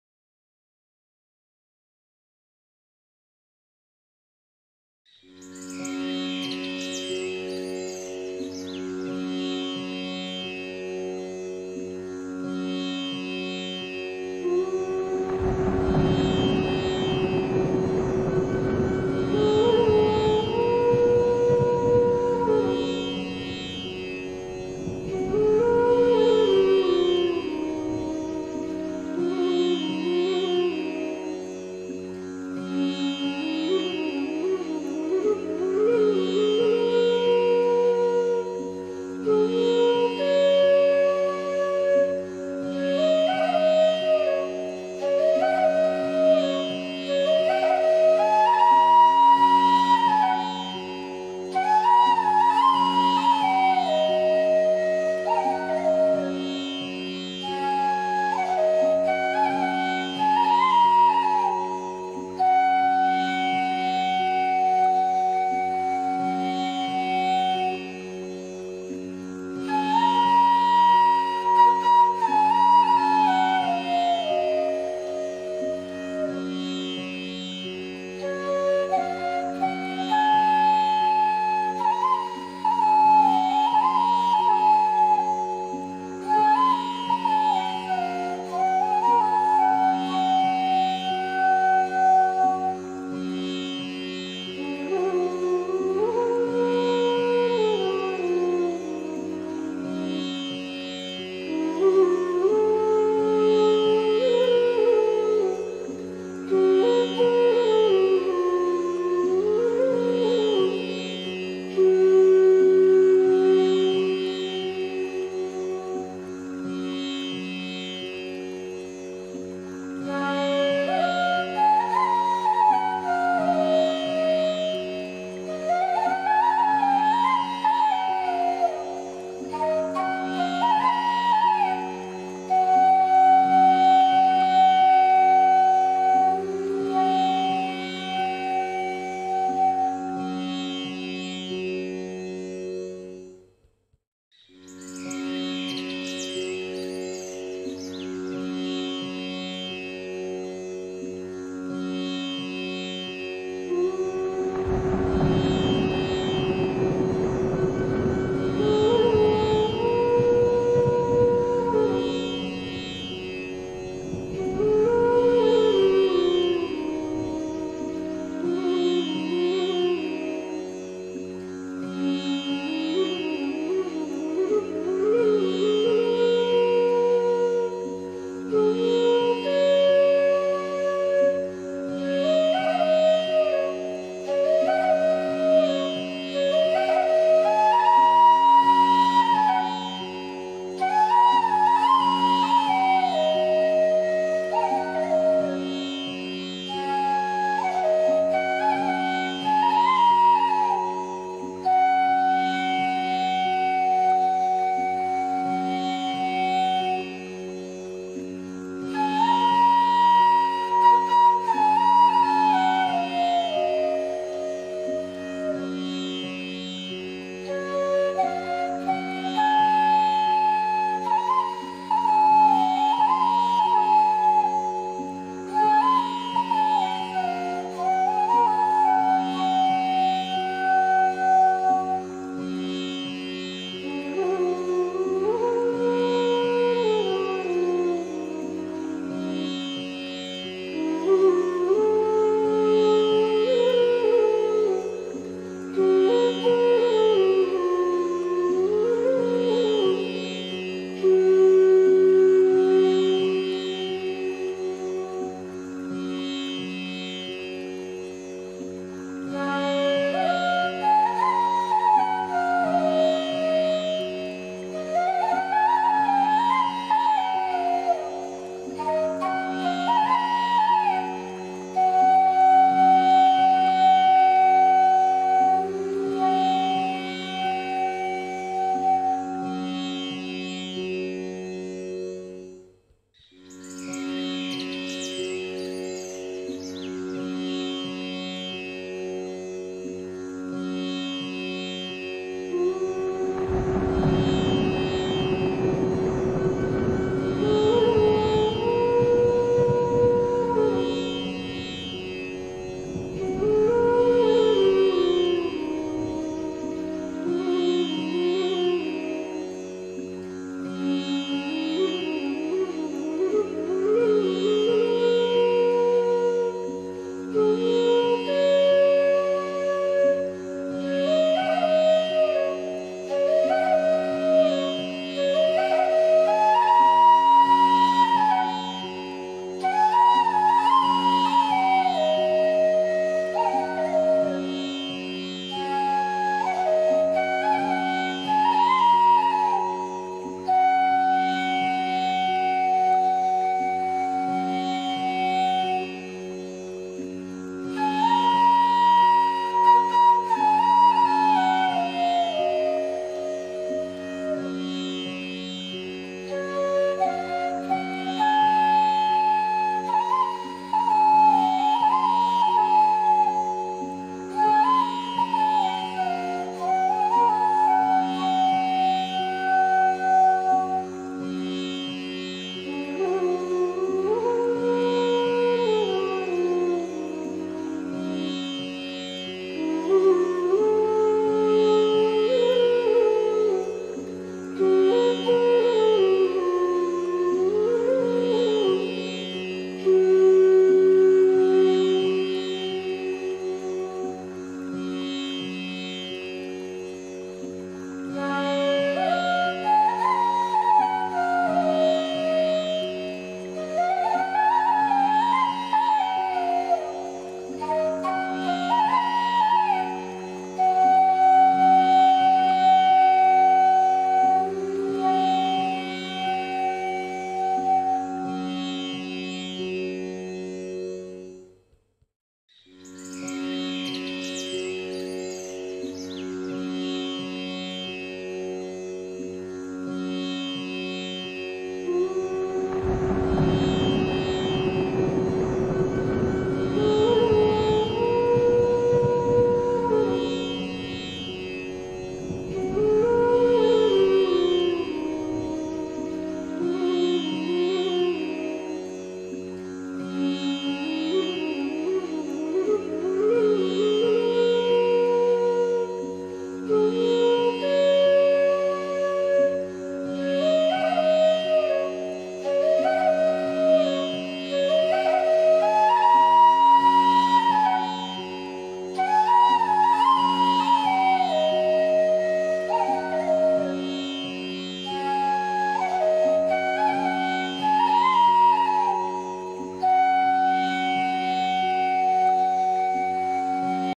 Lord krishna drawing with oil pastel - step by step for beginners ||dhruv kumar Arts
lots of ❤